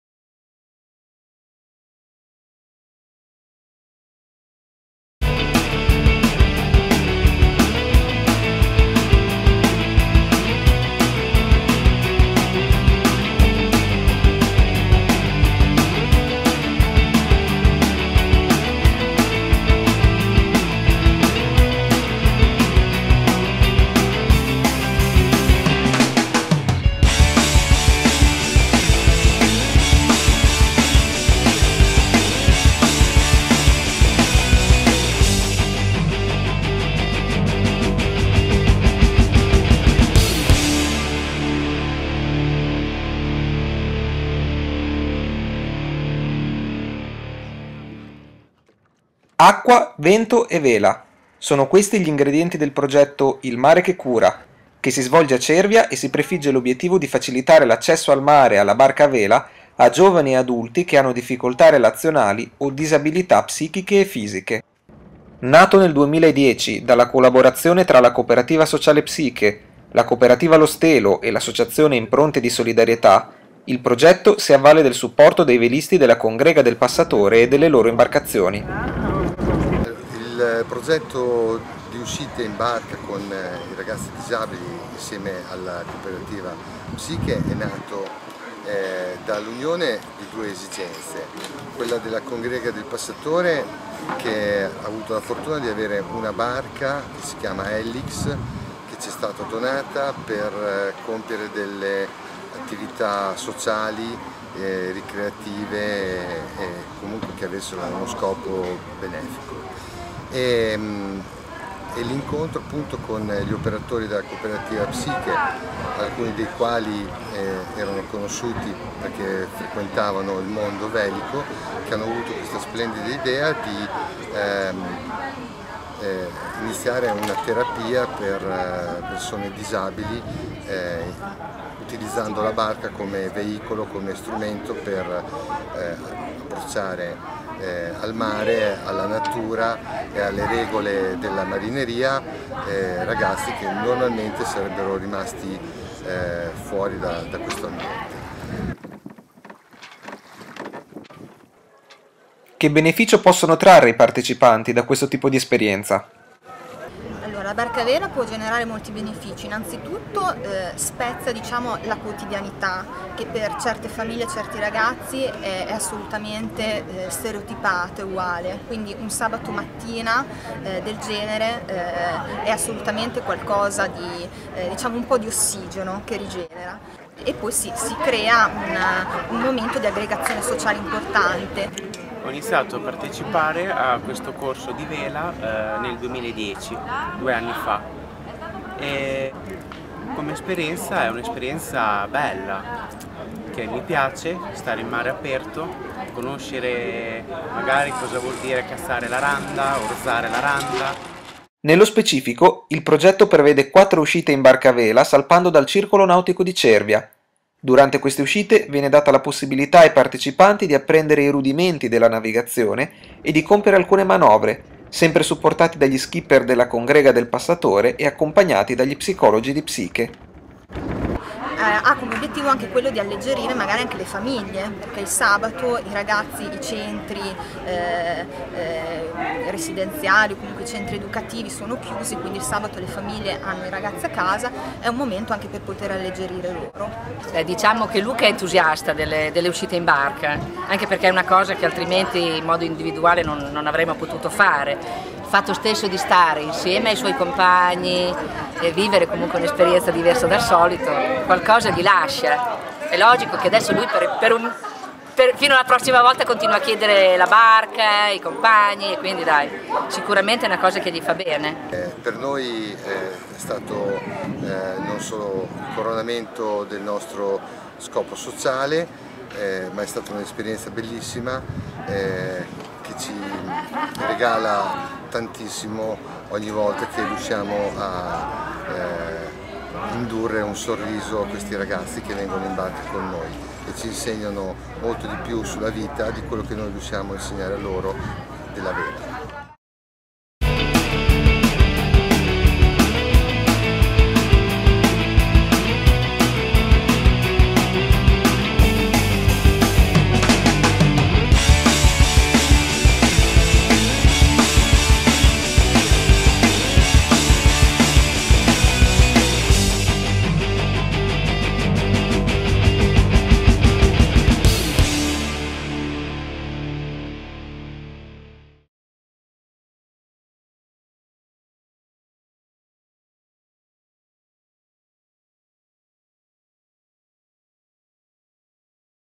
Il mare che cura
Cervia, 28 luglio 2012

Fare Comunità incontra organizzatori e partecipanti al progetto IL MARE CHE CURA, che utilizza la barca a vela come terapia per persone con disabilità fisiche e psichiche.

Iniziato nel 2010, il progetto è supportato dalla Pubblica Assistenza di Cervia, dai velisti della Congrega del Passatore e dal Circolo nautico di Cervia.

IL MARE CHE CURA, promosso dalla coop sociale Psicke, dall'associazione Impronte di Solidarietà e dalla coop sociale Lo Stelo, ha il supporto economico del Comune di Cervia, di Auxilia Onlus e dalla Banca di Cesena Credito Cooperativo.